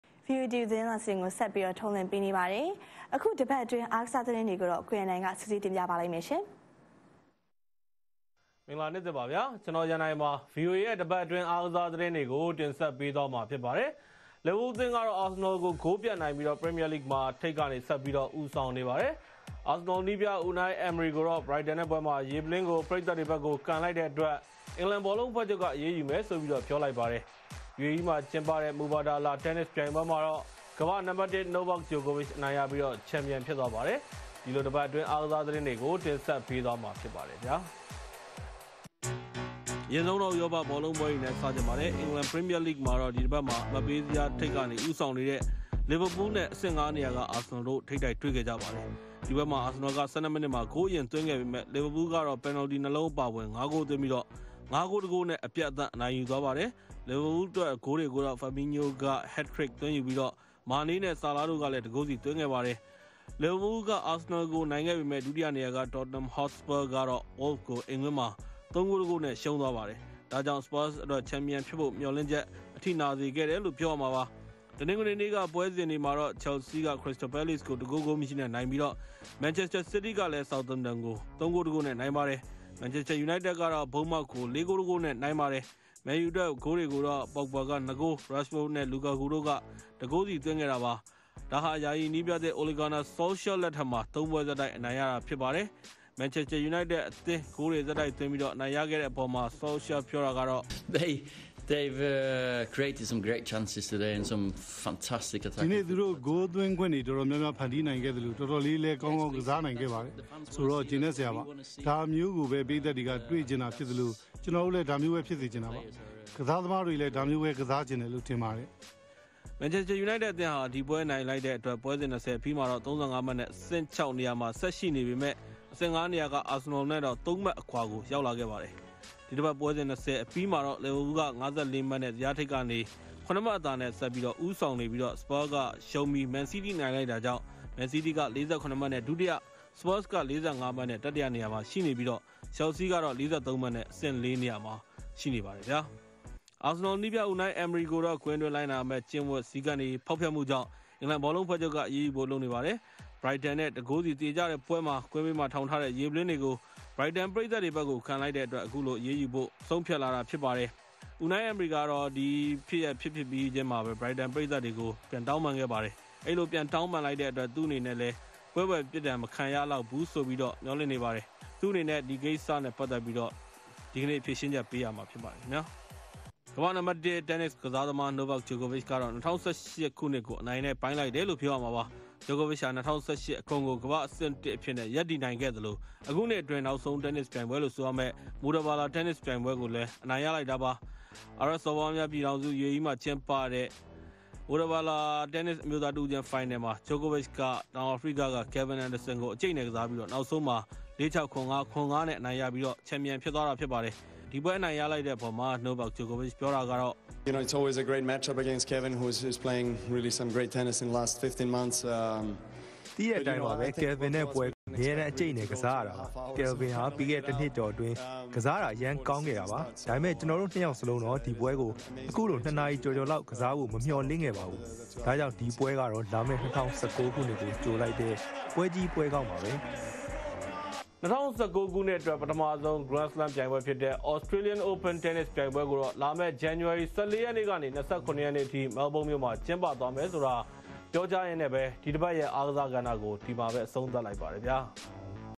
တပတ္အတြင္း အားကစား
အခု တပတ္အတြင္း အားကစားသတင္းေတြကို ကိုရန္ႏုိင္က စုစည္းတင္ျပပါလိမ့္မယ္။